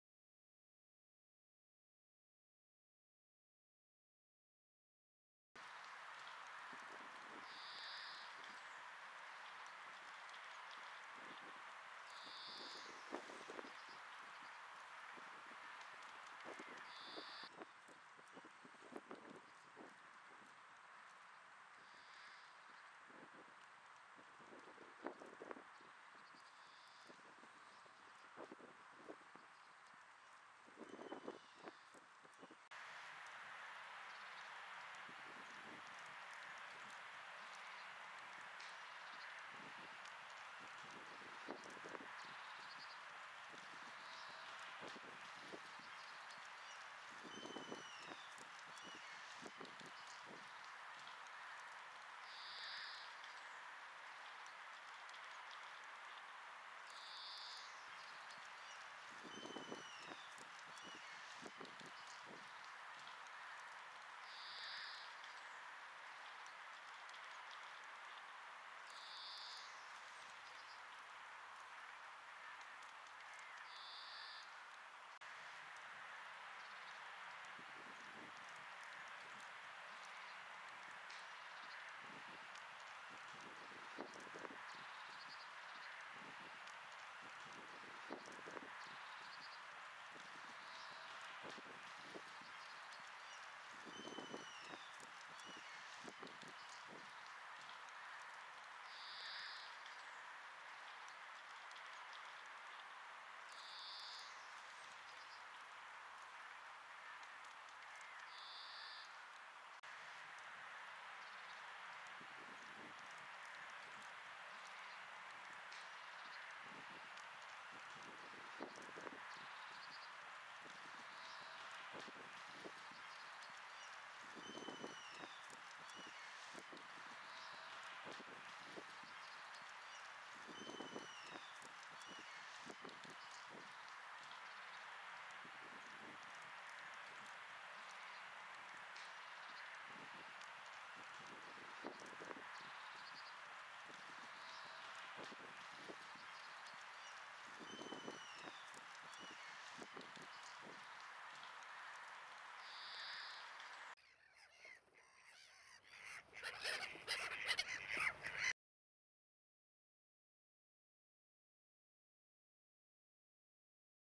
Vattensork
Vattensork vid Ormstaån i Vallentuna i  april 2011